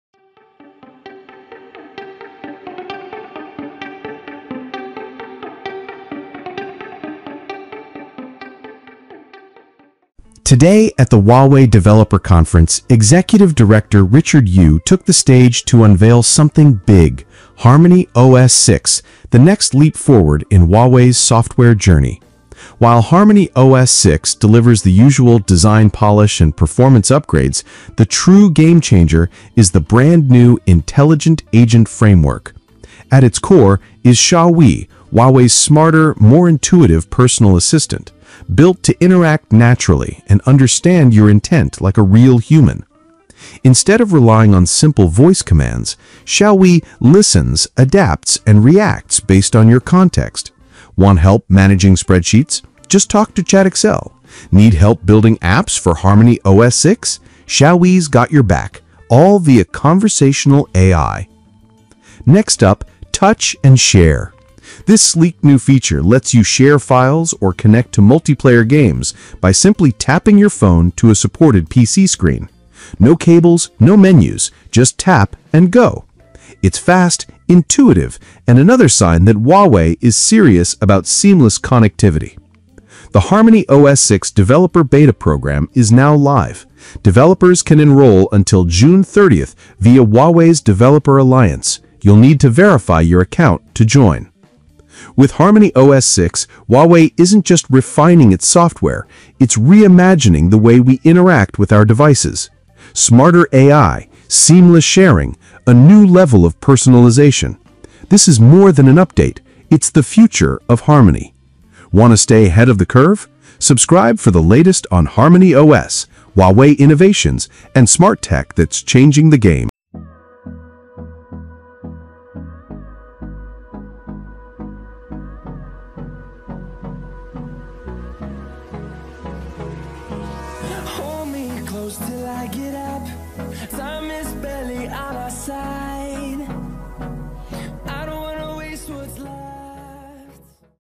Huawei unveiled HarmonyOS 6 packed with stunning upgrades, and the revolutionary XiaoYi AI assistant
Huawei just dropped a bombshell at the 2025 Developer Conference! 🚀 Executive Director Richard Yu unveiled HarmonyOS 6, packed with stunning upgrades, seamless device interaction, and the revolutionary XiaoYi AI assistant. From conversational app building to file sharing with a tap, this is not just an OS — it’s a glimpse into the future.

💡 Discover how XiaoYi’s intelligent agent framework is taking on Siri and Google Assistant.
🎮 Watch the magic of Touch and Share in action — no cables, no menus, just tap and go!
🧠 Smarter, faster, more intuitive — HarmonyOS 6 is rewriting the rules of mobile interaction.

🔥 Developer Beta is now LIVE until June 30! Don’t miss your chance to shape the future.